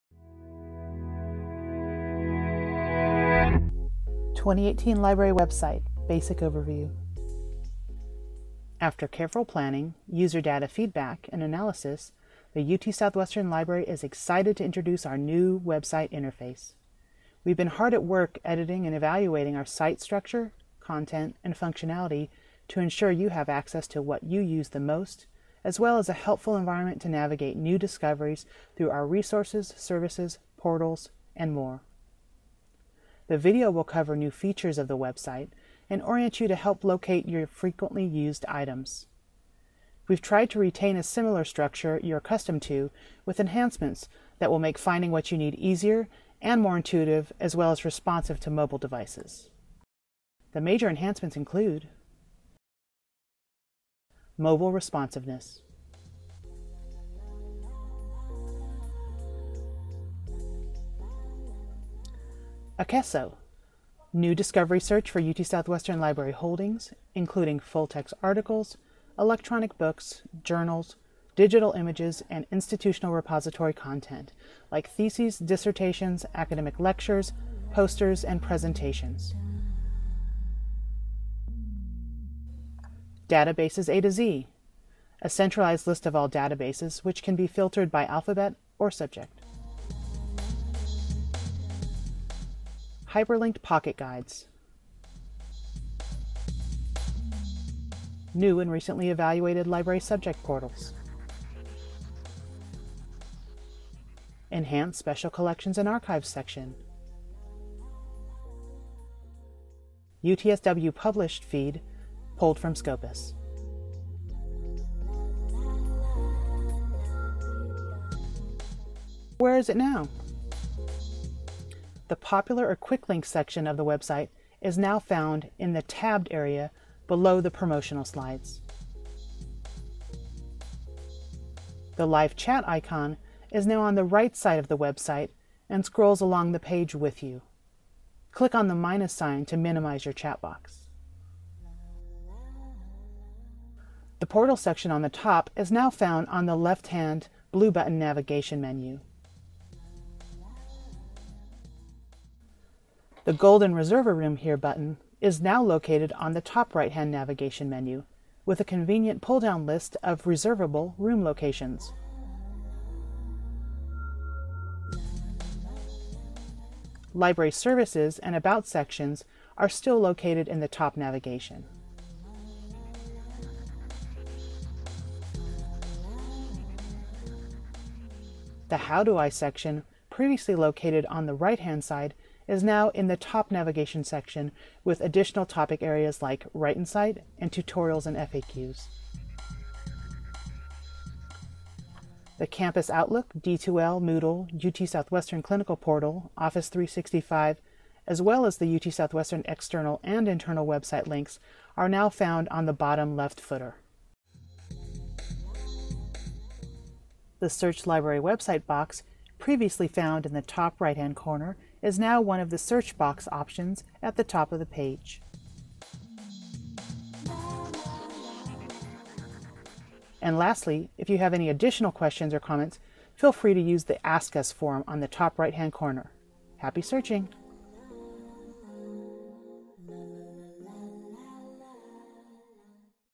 UT Southwestern Health Sciences Digital Library and Learning Center 2018 Website Overview
This is a brief overview of the UT Southwestern Health Sciences Digital Library and Learning Center's new website, which was launched in April 2018. It covers new features and provides a navigation overview in comparison with the previous site.